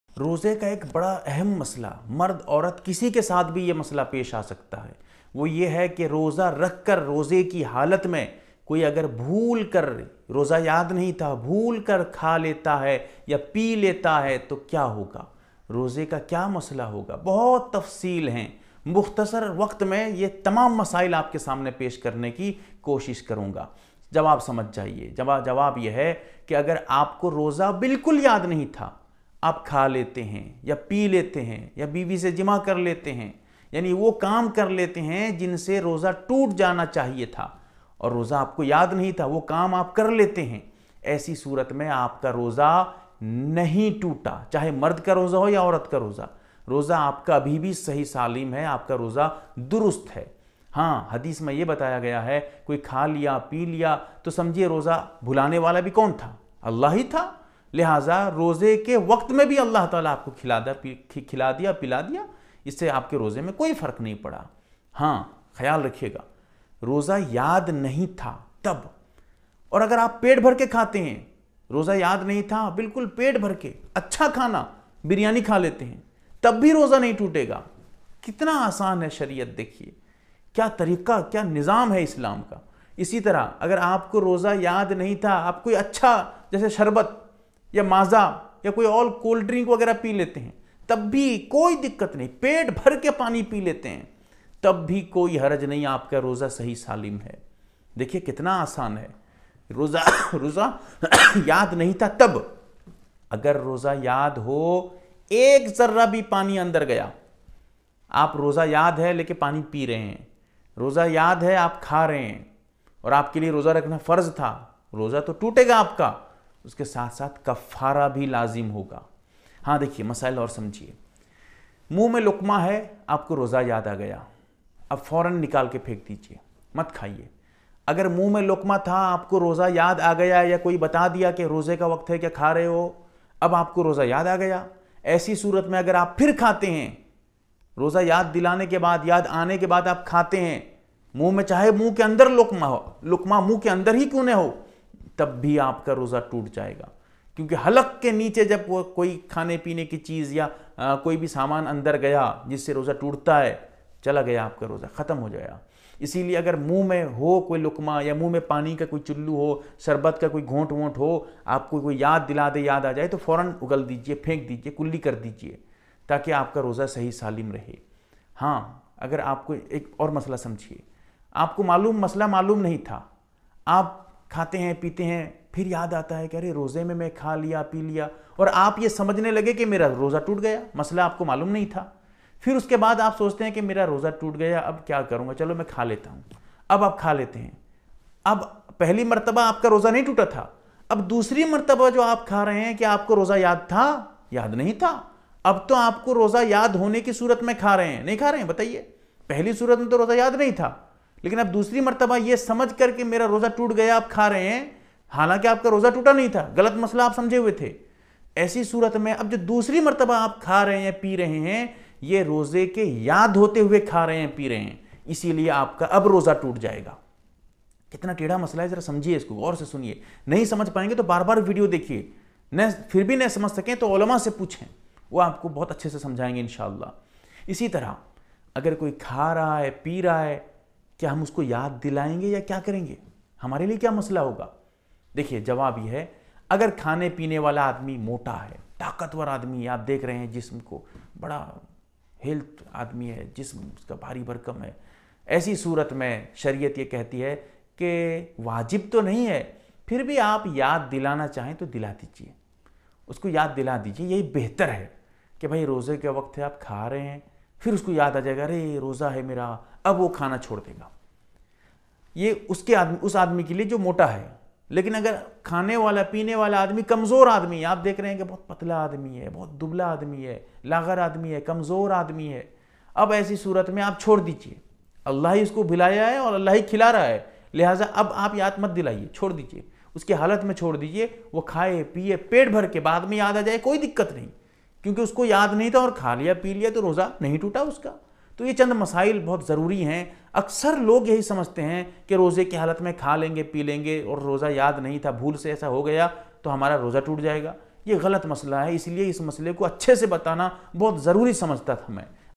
Roze Ki Halat Me Bhool Kar Kha Pee Len To Kya Hoga | Roze Me Bhool Kar Khana Peena Kaisa Hai | Roza
Roze ki halat me agar koi bhool kar kha pee leta hai to kya masla hoga? roze daar ko roza yaad nahi tha fir wo khana peena karta hai to kya hoga important masail full details by Mufti Abdur Rahman Banahara